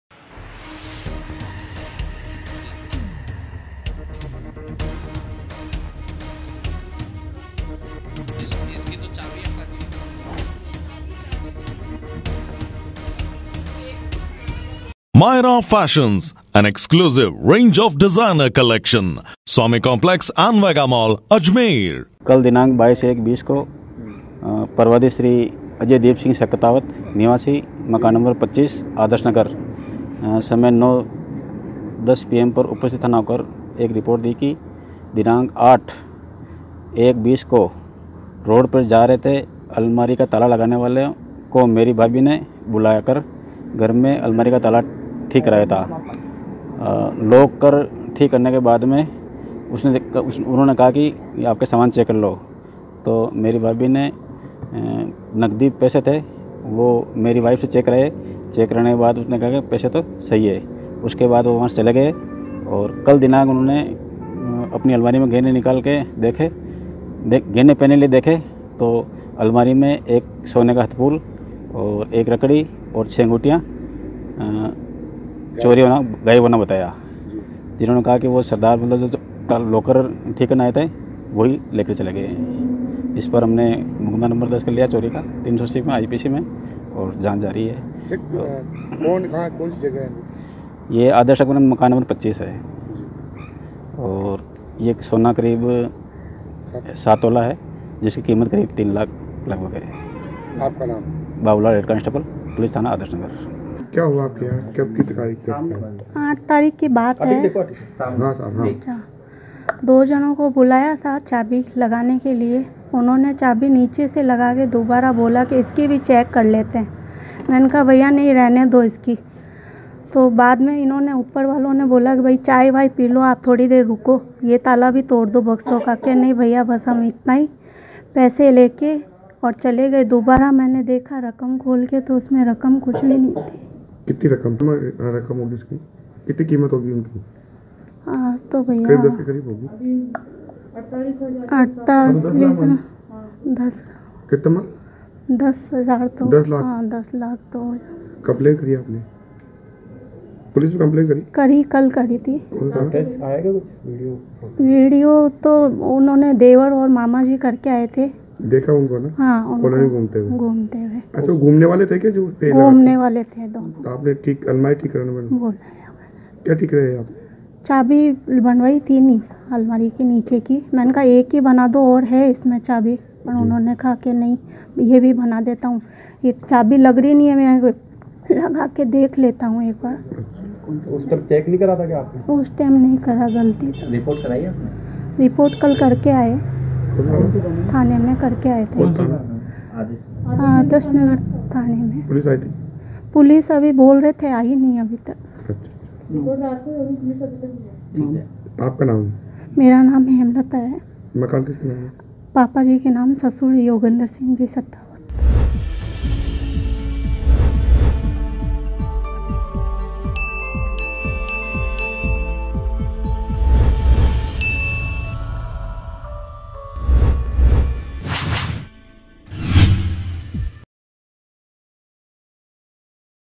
AJMER NEWS | आदर्श नगर निवासी शेखावत की बक्से में रखा 7 तोला सोना चोरी | MTTV INDIA
बक्से में रखा 7 तोला सोना की चोरी,ताले की चाभी बनाने आए बदमाशों ने दिया वारदात को अंजाम,पीड़ित योगेंद्र सिंह शेखावत ने आदर्श नगर थाने में दी शिकायत,चाबी बनाते समय परिजनों ने नहीं रखा चोरों पर ध्यान,आदर्श नगर निवासी शेखावत की शिकायत पर पुलिस जुटी फुटेज तलाशने में